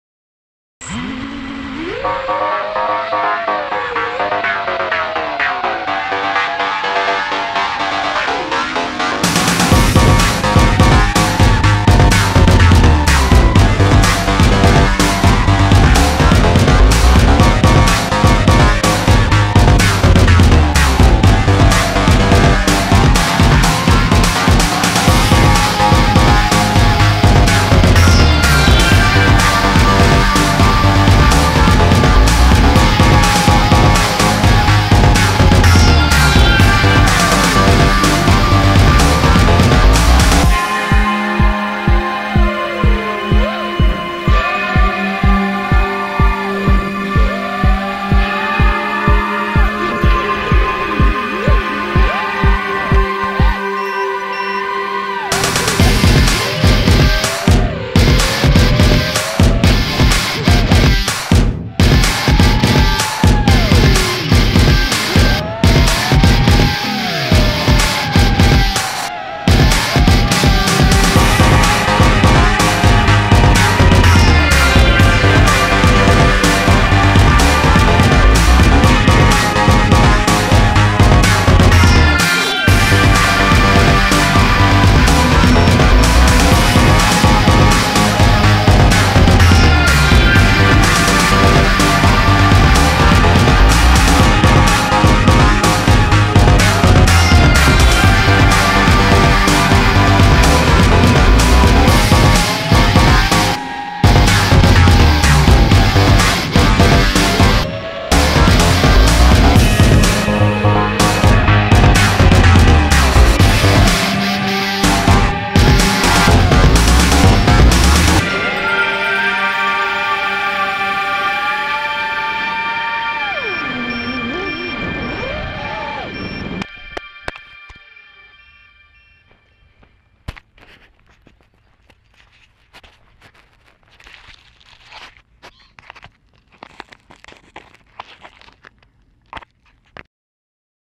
FPV new tricks
Getting some tricks down. First yaw spin while vertical. Yay
And some crashes

Lumenier QAV180
Naze32 Betaflight airmode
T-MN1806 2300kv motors
DYS 20amp esc
DAL 4x4.5x3 Bullnose props
Go Pro 3
Immersion 5.8 25mw
1300 mah Turnigy and Volo S4 Batts